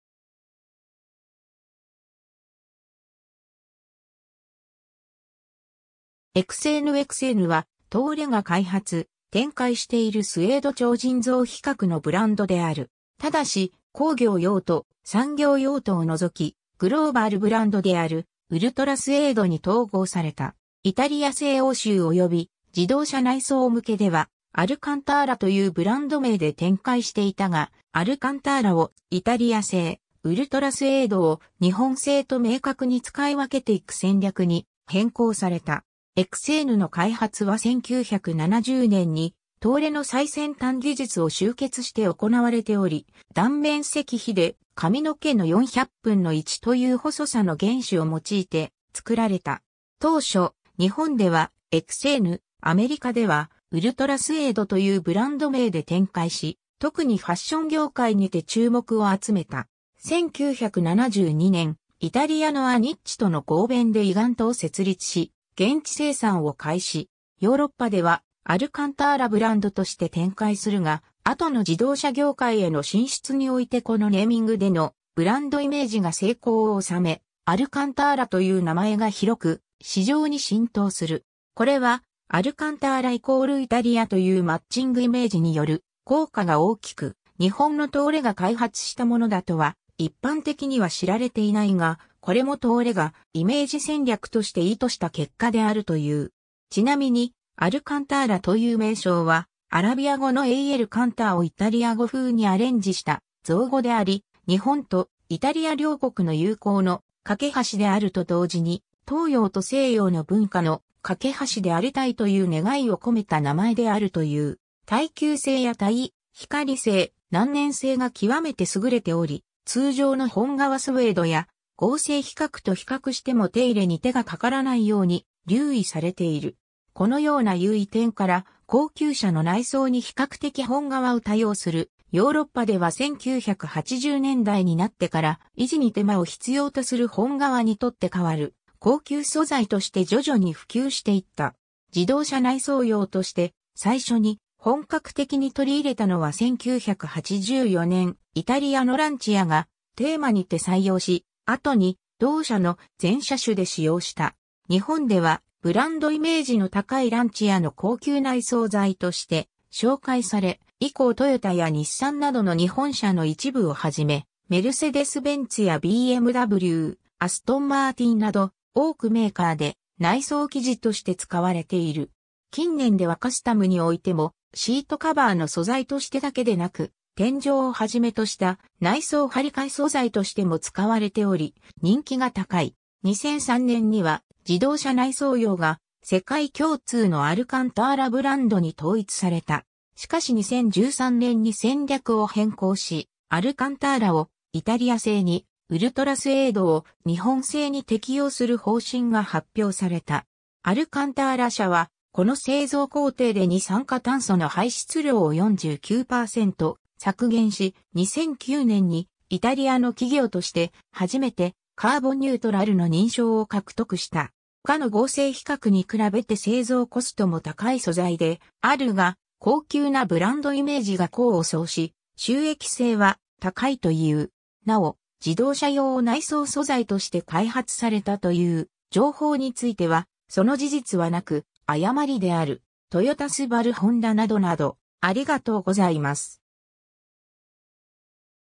エクセーヌ
エクセーヌ エクセーヌ（Ecsaine）は、東レが開発・展開しているスエード調人造皮革のブランドである。
ただし工業用途・産業用途を除きグローバルブランドであるウルトラスエード (Ultrasuede) に統合された。
イタリア製欧州及び自動車内装向けではアルカンターラ（Alcantara）というブランド名で展開していたが、アルカンターラをイタリア製、ウルトラスエードを日本製と明確に使い分けていく戦略に変更された。
 エクセーヌの開発は1970年に東レの最先端技術を集結して行われており、断面積比で髪の毛の400分の1という細さの原糸を用いて作られた。
 当初、日本ではエクセーヌ、アメリカではウルトラスエードというブランド名で展開し、特にファッション業界にて注目を集めた。
1972年、イタリアのアニッチとの合弁でイガント（現在のアルカンターラ社）を設立し現地生産を開始。
ヨーロッパではアルカンターラブランドとして展開するが、後の自動車業界への進出においてこのネーミングでのブランドイメージが成功をおさめ、アルカンターラという名前が広く市場に浸透する。
これはアルカンターラ＝イタリアというマッチングイメージによる効果が大きく、日本の東レが開発したものだとは一般的には知られていないが、これも東レがイメージ戦略として意図した結果であるという。
ちなみに「アルカンターラ」という名称は、アラビア語のAl Kantarをイタリア語風にアレンジした造語であり、日本とイタリア両国の友好の「架け橋」であると同時に、東洋と西洋の文化の「架け橋」でありたいという願いを込めた名前であるという。
 耐久性や耐光性・難燃性が極めて優れており、通常の本革スウェードや合成皮革と比較しても手入れに手がかからないように留意されている。
このような優位点から、高級車の内装に比較的本革を多用するヨーロッパでは1980年代になってから、維持に手間を必要とする本革にとって代わる高級素材として徐々に普及していった。
自動車内装用として最初に本格的に取り入れたのは1984年、イタリアのランチアがテーマにて採用し、後に同社の全車種で使用した。
日本ではブランドイメージの高いランチアの高級内装材として紹介され、以降トヨタや日産などの日本車の一部を始め、メルセデス・ベンツやBMW、アストンマーティンなど、多くメーカーで内装生地として使われている。
近年ではカスタムにおいても、シートカバーの素材としてだけでなく天井を始めとした内装張替え素材としても使われており、人気が高い。
 2003年には自動車内装用が世界共通のアルカンターラ・ブランドに統一された。
しかし2013年に戦略を変更し、アルカンターラをイタリア製に、ウルトラスエードを日本製に適用する方針が発表された。
 アルカンターラ社は、この製造工程で二酸化炭素（CO2）の排出量を49%削減し、2009年にイタリアの企業として初めて「カーボンニュートラル」の認証を獲得した。
他の合成皮革に比べて製造コストも高い素材であるが、高級なブランドイメージが功を奏し収益性は高いという。
なお、「自動車用内装素材として開発された」という情報についてはその事実はなく、誤りである。
 トヨタ スバル ホンダ など など